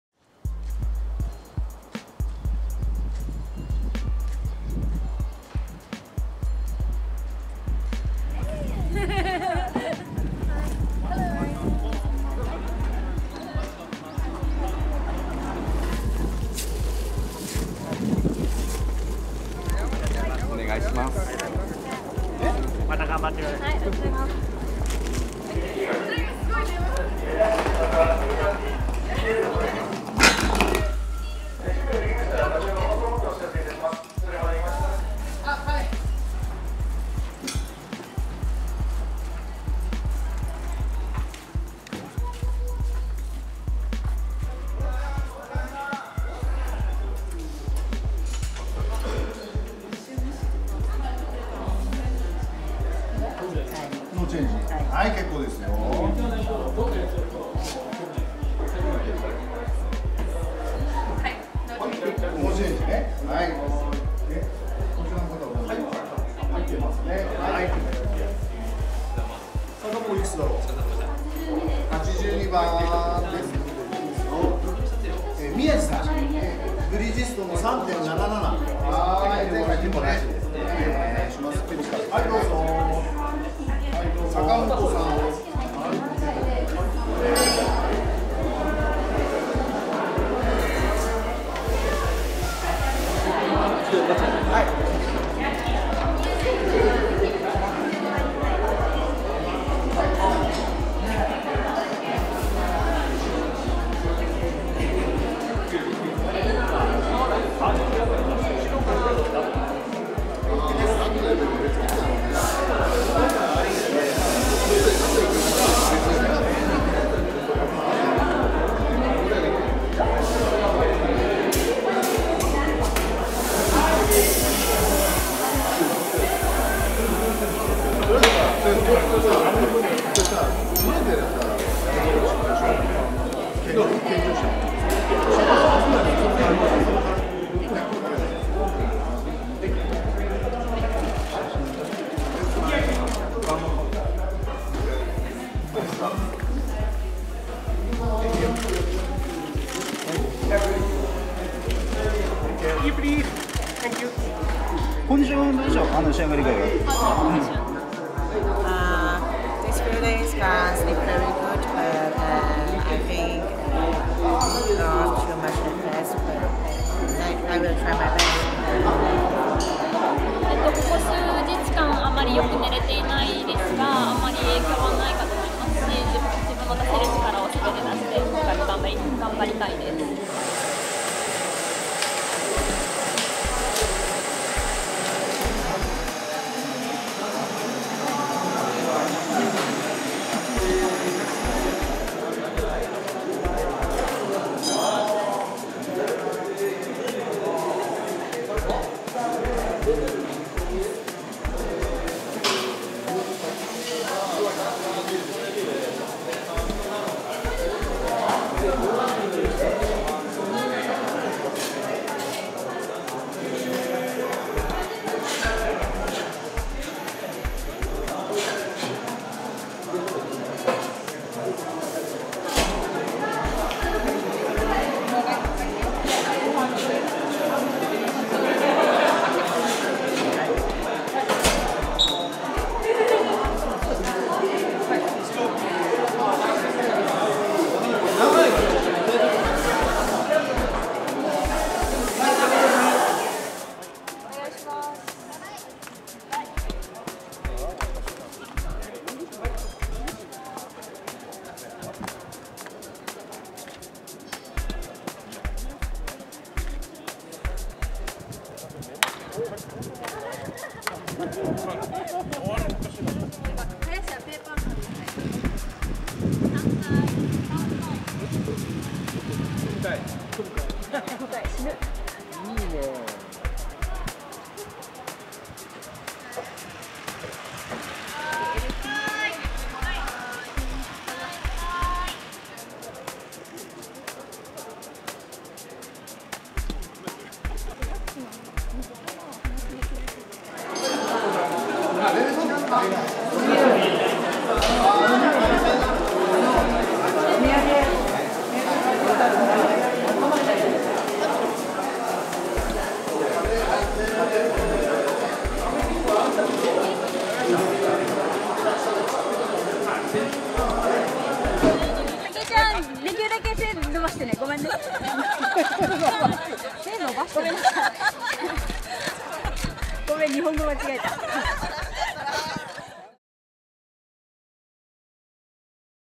03.Nov.2016 女子短期登録最終戦 京王閣前検日
2016年11月3日、2016年の女子短期登録選手最終戦の前検日の模様です。
撮影編集・丸橋利城　KEIRIN MAGAZINE